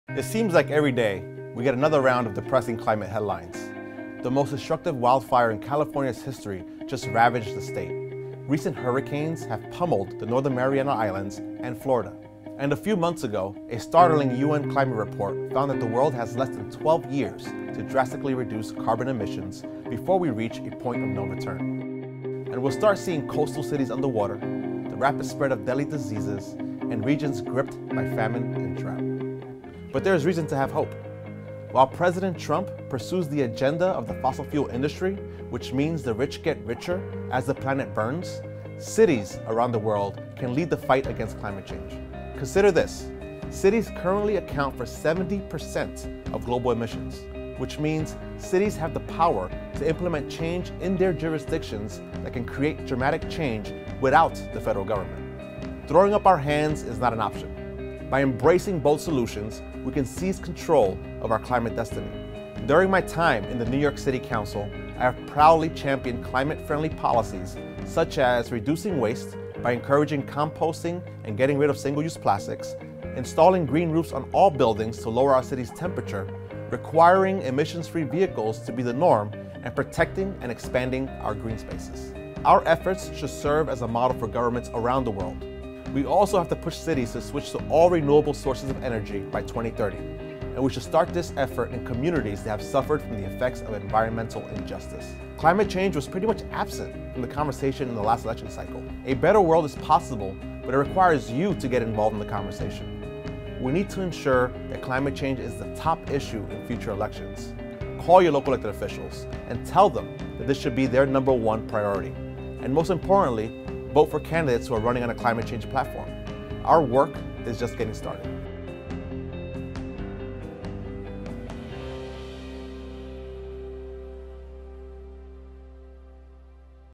Why Politicians Need To Run On Climate Change | Op-Ed | NowThis
We can't give up on climate change prevention just because of who's in the White House — here's how cities can act now to save our planet, according to NYC Council Member Rafael L. Espinal Jr.

In the face of increasing wildfires, hurricanes, droughts, and reports that warn of even more dire consequences of climate change, NYC Council Member Rafael L. Espinal Jr. says cities can do their part to roll back the tide of global warming. 'Throwing up our hands is not an option,' Espinal says, instead cities could embrace bold solutions without federal government action. WE can reduce waste by encouraging composting, getting rid of single-use plastics, installing green roofs on buildings, requiring emissions-free vehicles, and protecting and expanding green spaces. This city-level efforts can serve as a model for governments around the world.